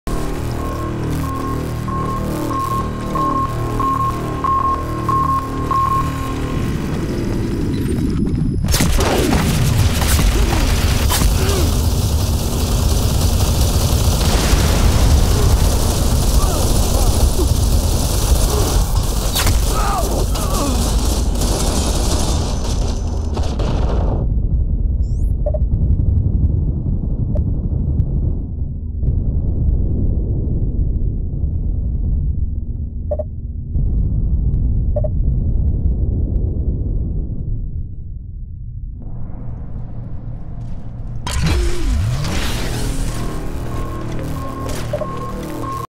How to Ruin Aegis Like a boss
Moving my team up field when the other team brought out a Great can of whoop ass. .